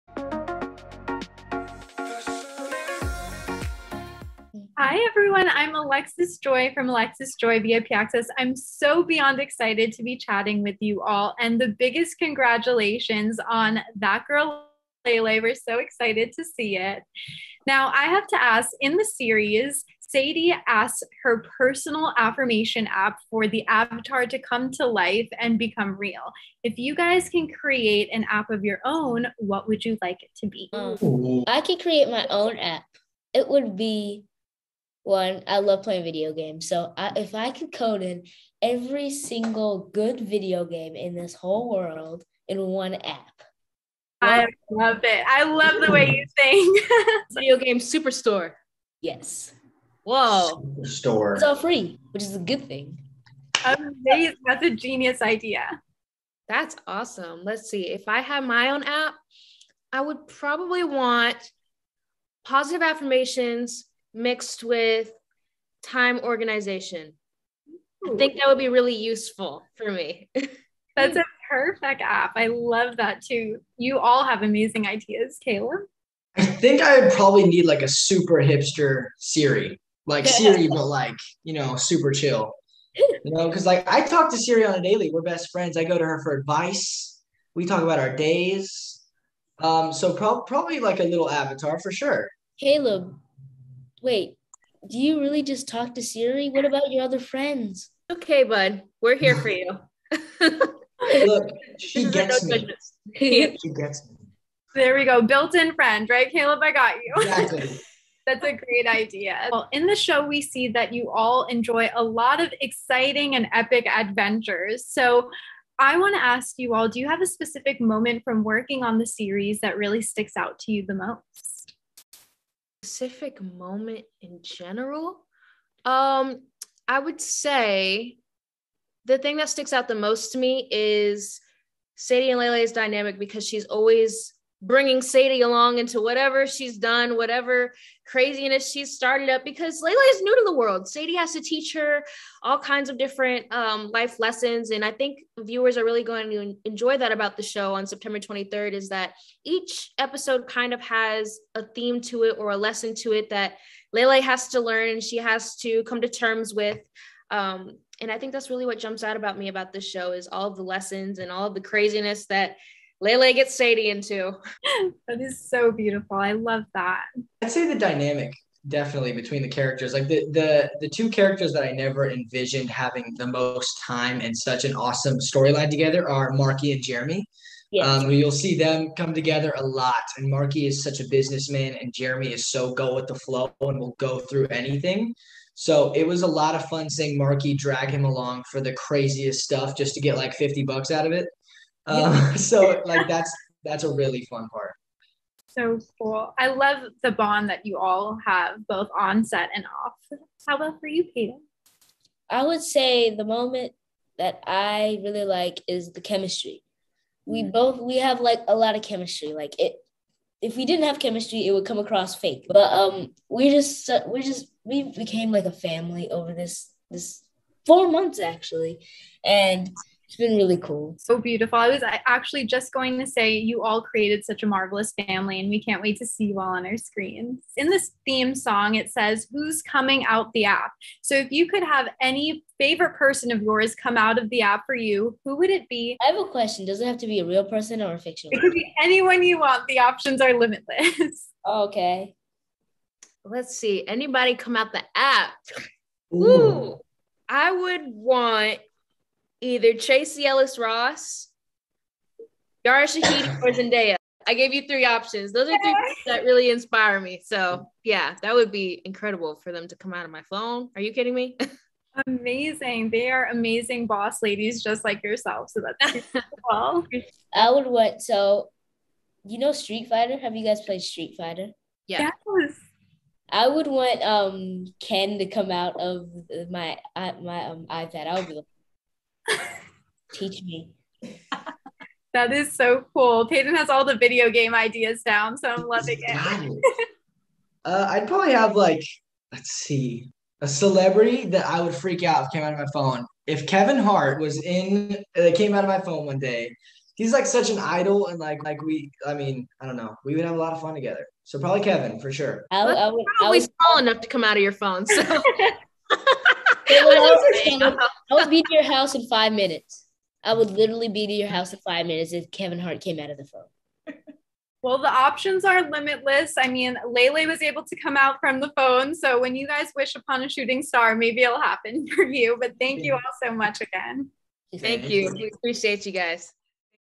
That Girl Lay Lay Cast Interview - Gabrielle Nevaeh Green, Caleb Brown & Peyton Perrine III
She's coming out the app and coming onto our TV screens - say it with us... it's That Girl Lay Lay! In Nickelodeon's highly-anticipated, buddy comedy series, "That Girl Lay Lay," high-schooler Sadie (portrayed by the brilliant Gabrielle Nevaeh Green) has her own built-in bestie named Lay Lay, the avatar in her personal affirmation app. But, when you wish upon a shooting star... wishes do indeed come true as Sadie wishes for Lay Lay to be real. In the blink of an eye, Lay Lay comes to life and gets to navigate the real world, everything including high-school and hilarious hijinks, with Sadie right by her side.

A terrific series revolving around the power of female friendship and an endless amount of fun, "That Girl Lay Lay" is bound to be everyone's new favorite show to watch and enjoy. The marvelous series is comprised of an all-star, spectacular cast of gifted actors, who bring their amazing charm and talents so effortlessly to the show. The outstanding sitcom stars none other than Lay Lay herself, "All That" and "Unfiltered" star, Gabrielle Nevaeh Green, Peyton Perrine III, Caleb Brown, Tiffany Daniels, and Thomas Hobson. From behind-the-scenes photos that the cast shares on social media to seeing their exceptional work on our screens, it's clear to see that this phenomenal cast has created a fantastic family with a bond that can never be broken. The entire cast's great acting skills brings so much joy and merriment to viewers and we truly can't get enough of this incredible dream team.

"In the premiere episode of "That Girl Lay Lay," Sadie wishes upon a star that her hype-girl avatar, Lay Lay, were real, causing Lay Lay to magically come to life. Now that she is in the real world with Sadie, Lay Lay decides that she wants a teenage experience herself and enrolls in the local school. Using her innate charm, Lay Lay convinces Sadie that she needs to come out of her shell and the best way for her to stand out is to run for student council" (source: press release).

In each of the magnificent episodes, viewers will learn a lot of stellar and heartfelt, moral lessons, see the fun-loving camaraderie between Jeremy and Marky, and see how when Sadie and Lay Lay are together, they can truly take over the world!

AlexisJoyVIPAccess had a remarkable time interviewing Nickelodeon's "That Girl Lay Lay" stars Gabrielle Nevaeh Green, Caleb Brown and Peyton Perrine III all about something that sticks out to them the most from working on the show, who is someone they'd love to see come out from their phone and so much more. Plus, they also revealed what kind of app they would like to create themselves - hear what they had to say in our VIPAccessEXCLUSIVE interview!

Check out my VIPAccessEXCLUSIVE interview with "That Girl Lay Lay" stars Gabrielle Nevaeh Green, Peyton Perrine III and Caleb Brown and be sure to activate BFF mode and tune in to the series premiere of "That Girl Lay Lay" on Thursday, Sept. 23 at 7:30 PM ET / PT on Nickelodeon!